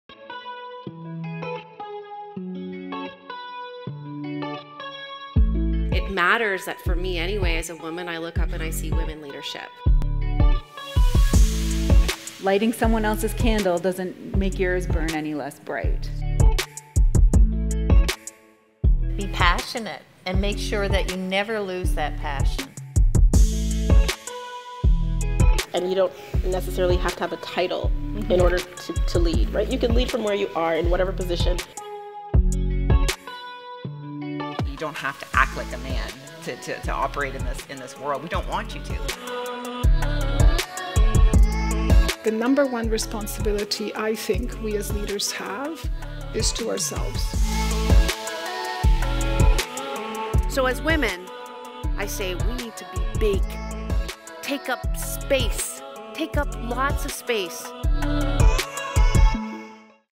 Whitby Chamber 2022 Connect Women Event
Client: Whitby Chamber of Commerce
Location: Whitby, Ontario
Decription: Highlights of the 2022 Connect Women: Leadership Redefined Event.